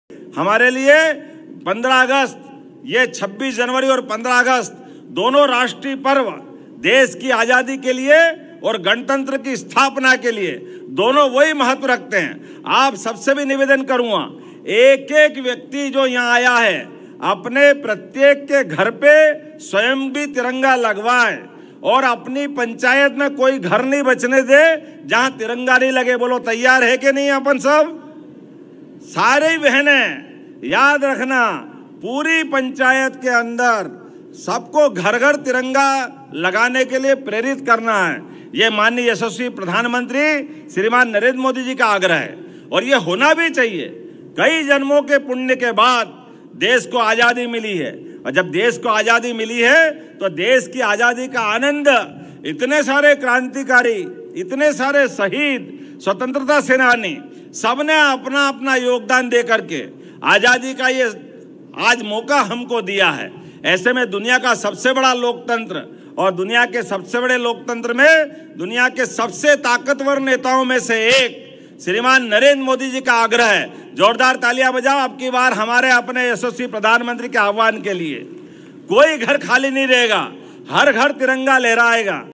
सभी से निवेदन है कि आप अपने घरों पर तिरंगा फहराएं : मुख्यमंत्री डॉ. मोहन यादव
यशस्वी प्रधानमंत्री श्री नरेंद्र मोदी जी के आह्वान पर पूरे देश में हर_घर_तिरंगा अभियान चलाया जा रहा है।

सभी से निवेदन है कि आप अपने घरों पर तिरंगा फहराएं।

मुख्यमंत्री डॉ. मोहन यादव